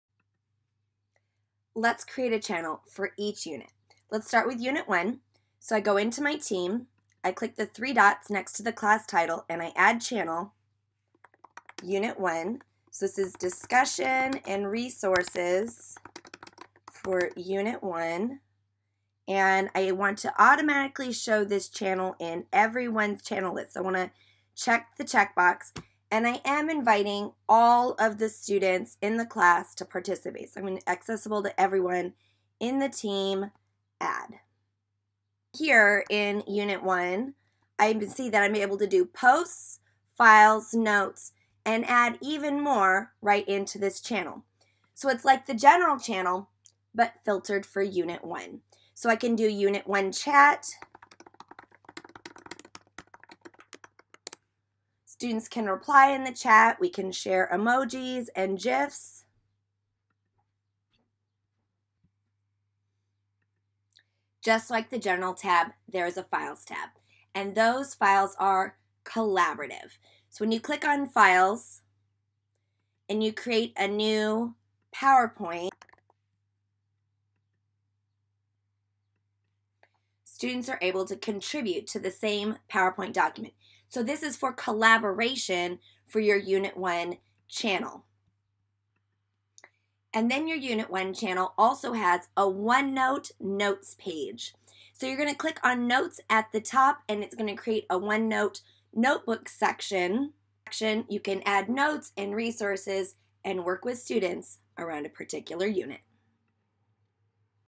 Creating a channel in Microsoft Teams for a Unit
For each unit, create a channel in Teams. This organizes your conversations and allows you to share a section of a OneNote Notebook.